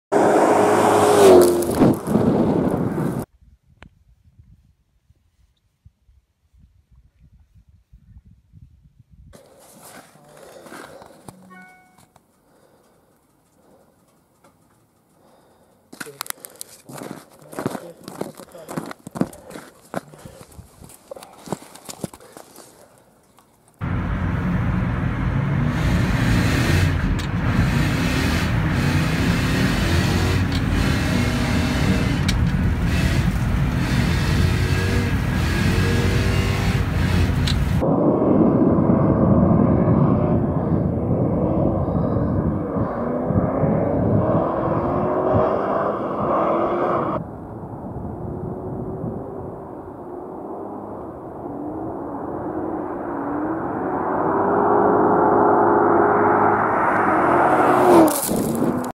BMW 540i V8 drifting on ice
BMW 540i E39 first testdrive on ice 2018 in Sweden.